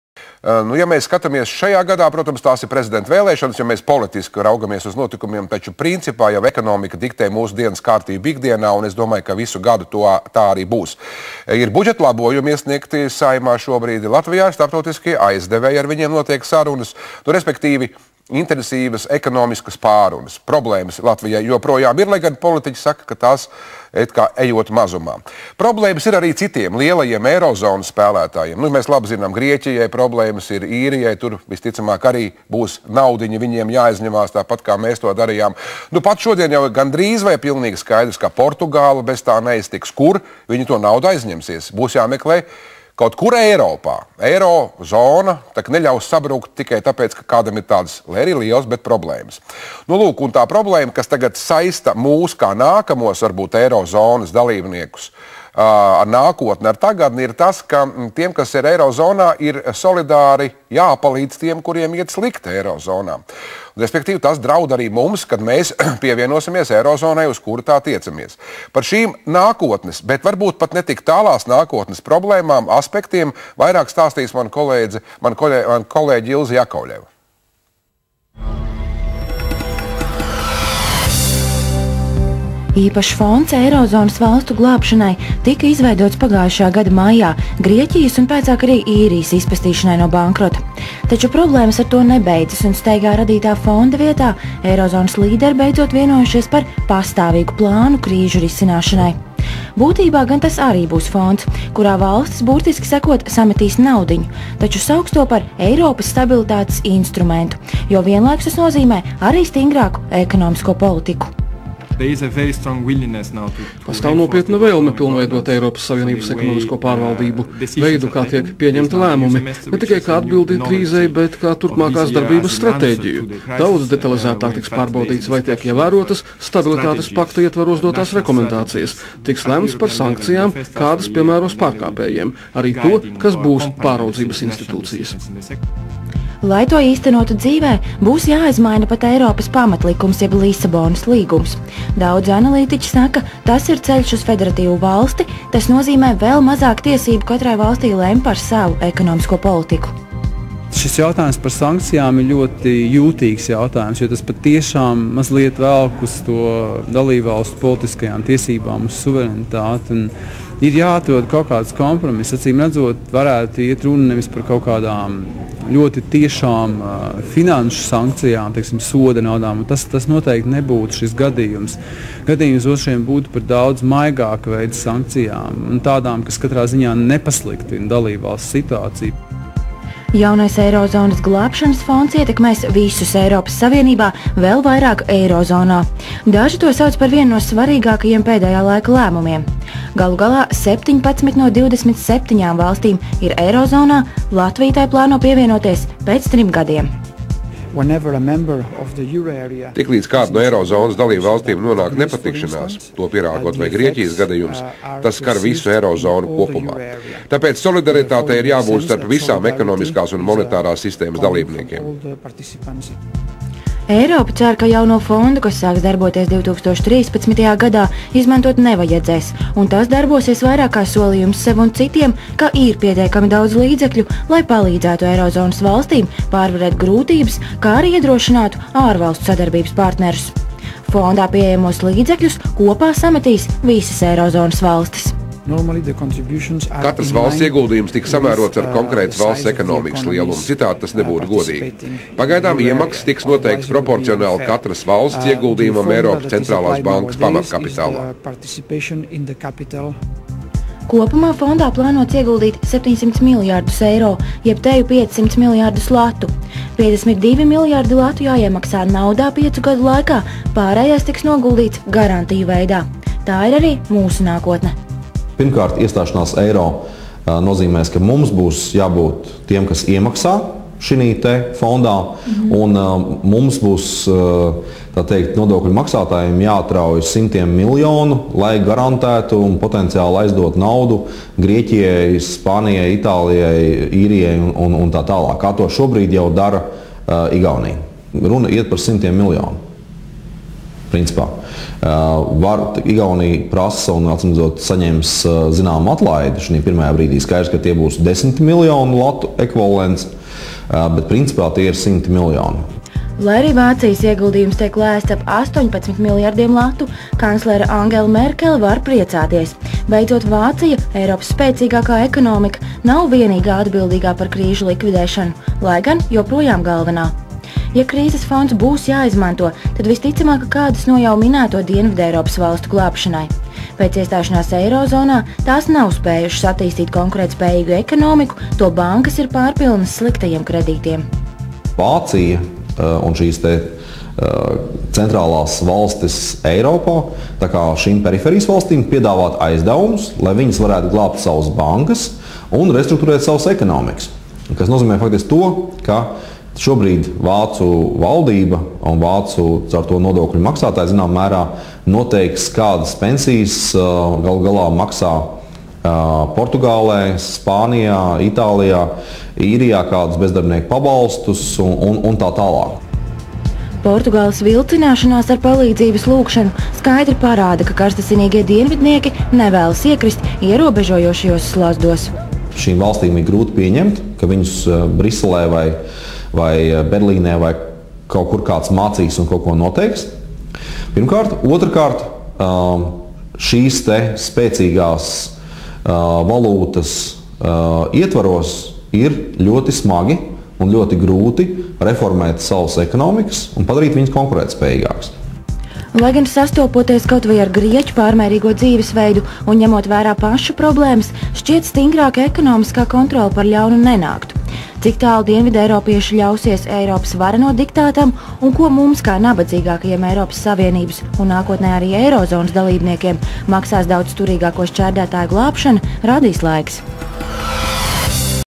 EIRO GLĀBĒJI
Tie, kas šodien pēdējie, rīt var kļūt ja ne par pirmie, tad vieni no glābējiem gan. Tiklīdz Latvija ieviesīs eiro, arī mums būs jāmaksā pārmērīgi tērējušo valstu glābšanas fondā. Un runa ir par simtiem miljonu latu. (JN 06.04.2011)